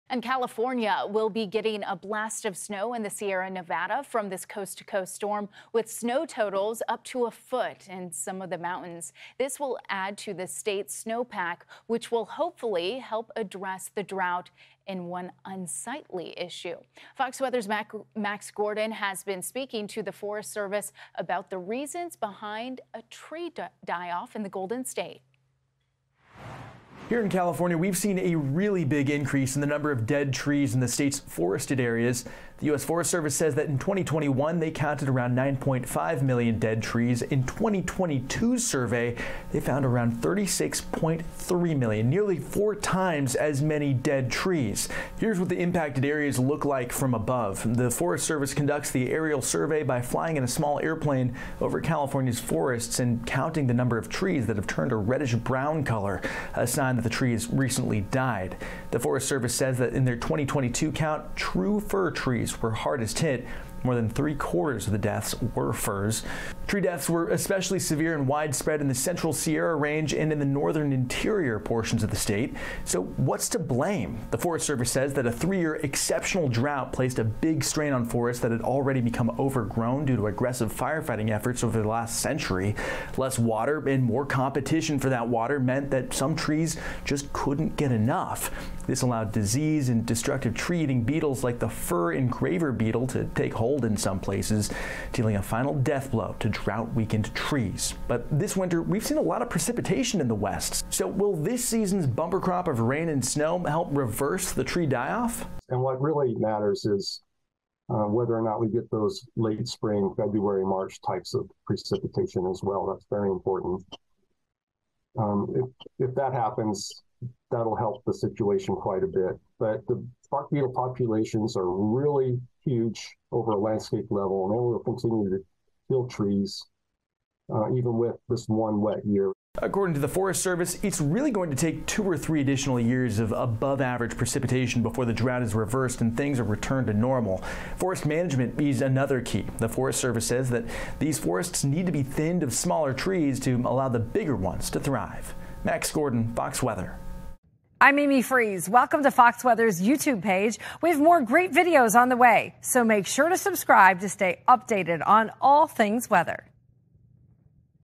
Millions of Trees Dying in California's Forests
Winter rains give the U.S. Forest Service hope that they can stem the mass die-off of trees in California's forests. FOX Weather correspondent Max Gorden explains how the years of drought have killed so many trees.